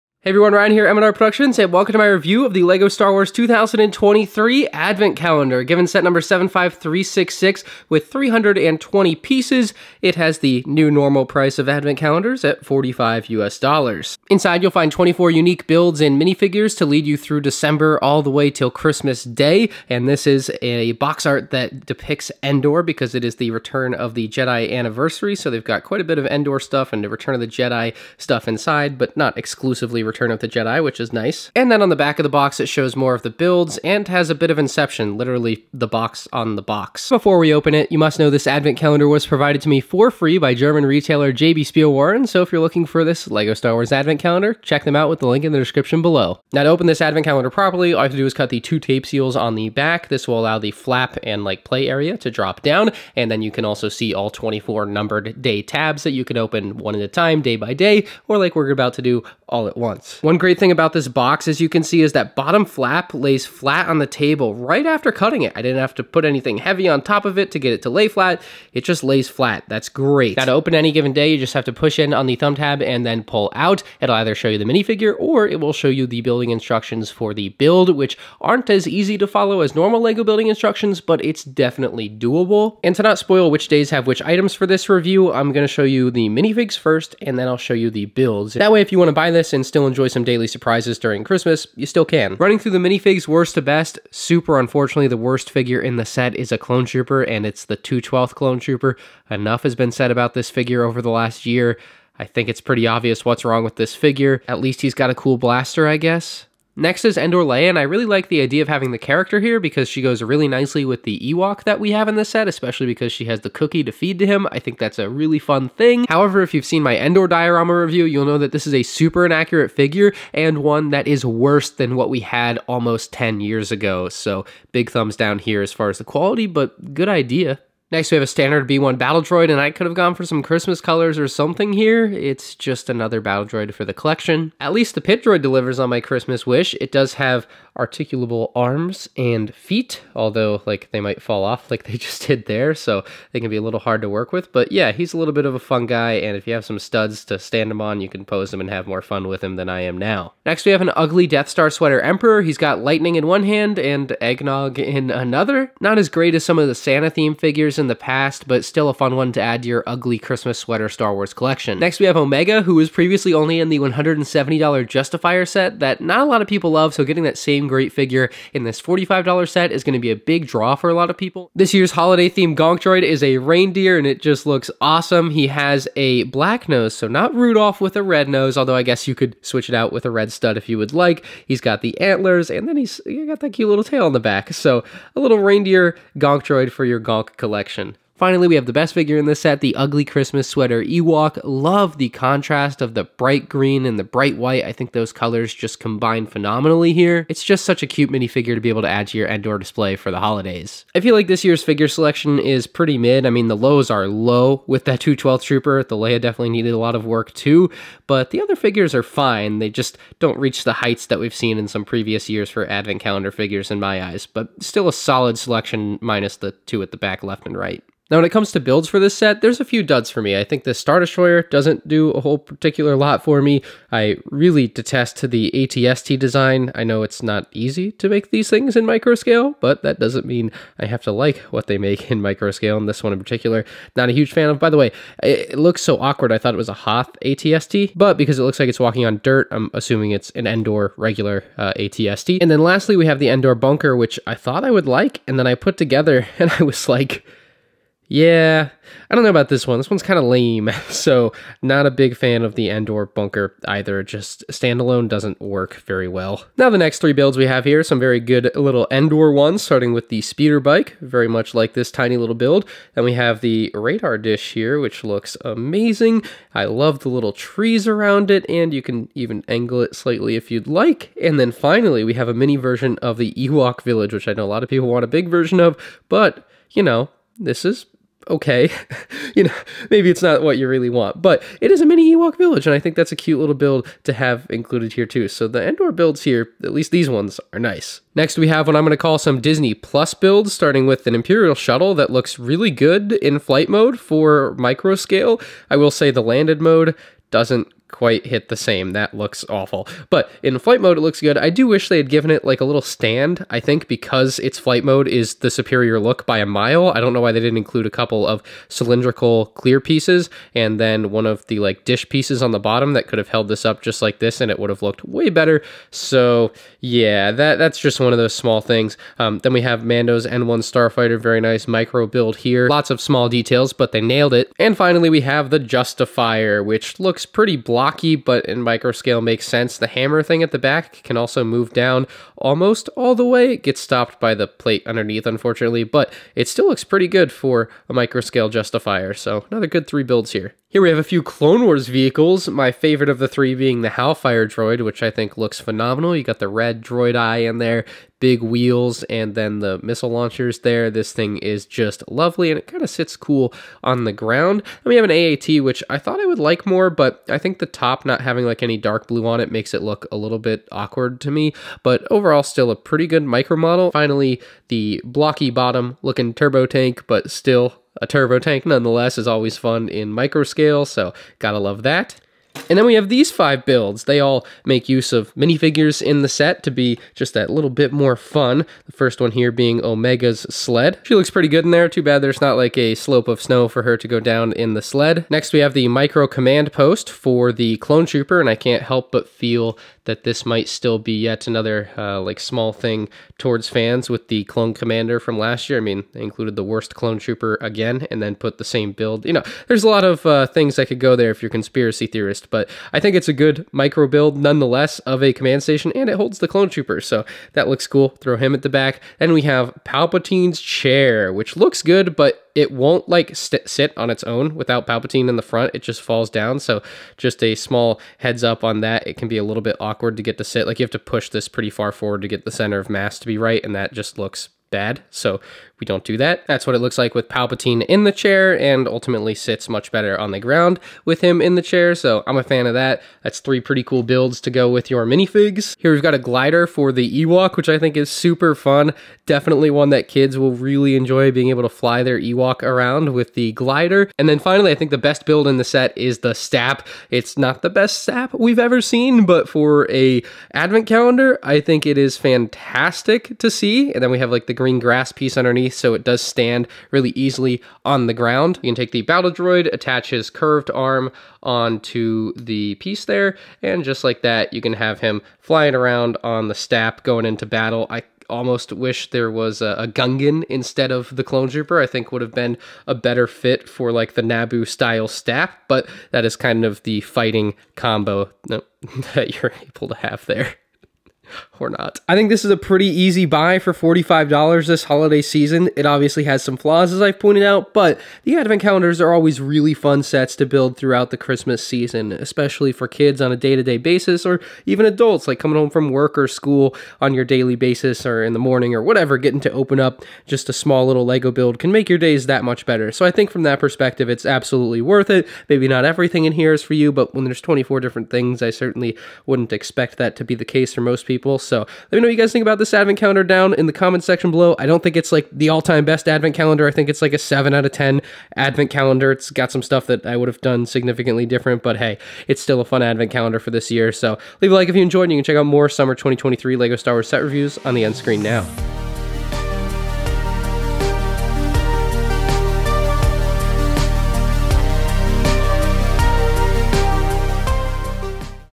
LEGO Star Wars 75366 ADVENT CALENDAR Review! (2023)
The 2023 LEGO Star Wars Advent Calendar is mid. In a good way. We have new holiday EWOK & EMPEROR PALPATINE Minifigures, fun builds like a STAP for your Battle Droid, Micro Clone Turbo Tank, Micro Mandalorian N-1, & more! But on the other end LEGO once again included an awful Clone Trooper with the 212th making an unfortunate appearance. LEGO Star Wars Advent Calendars are fun for the holidays and this will be no exception.

Thanks to JB Spielwaren (Toys)!